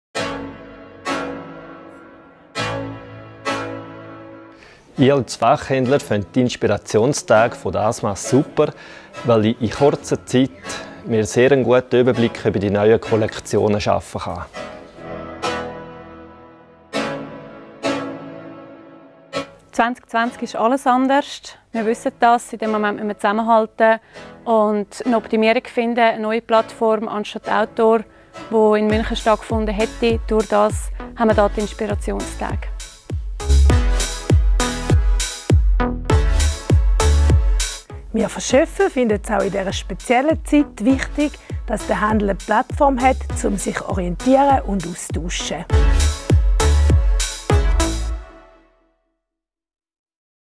inspirationstage sommer 2021 in Zürich
Am 29./30. Juni trifft sich Industrie und Handel des Schweizer Sportfachhandels in Zürich zu den ersten inspirationstage.